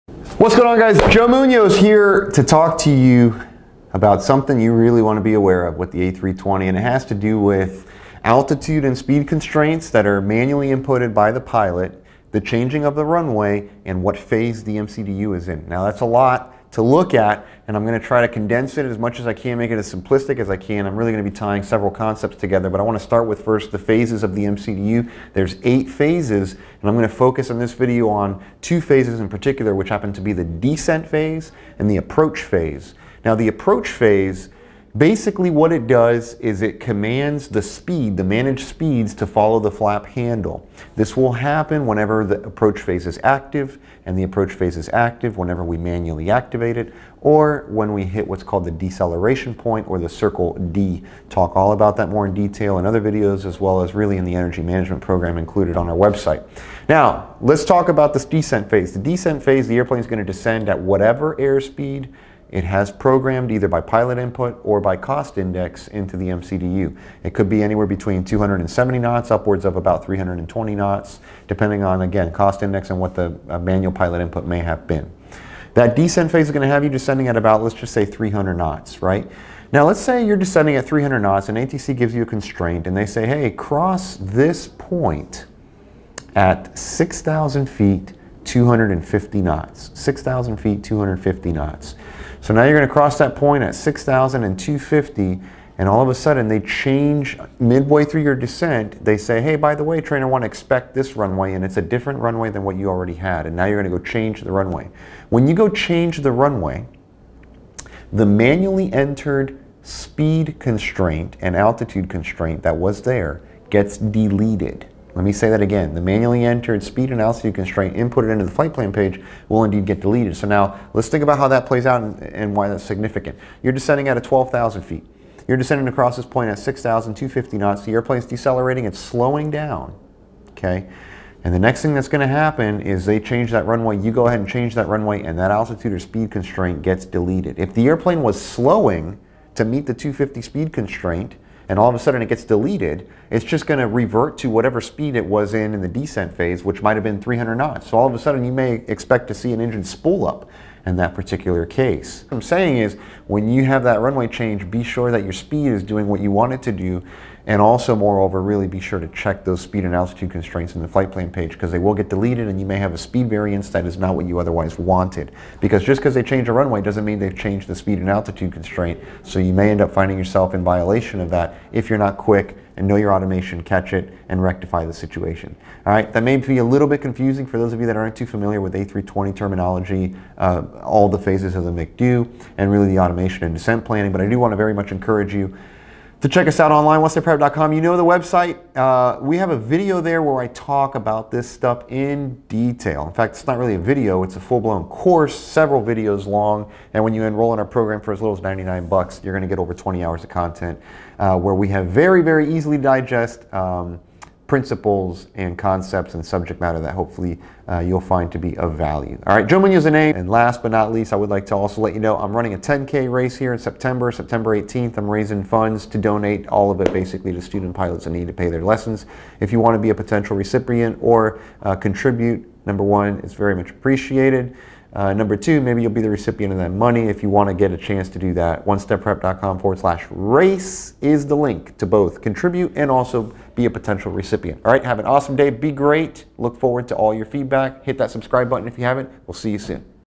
A320 Descent Trap!
Joe Munoz explains how changing runways un the descent phase can affect your MCDU FLT PLN Page!

Joe Munoz & Juan Dominguez
Professional Crew Training
Miami, FL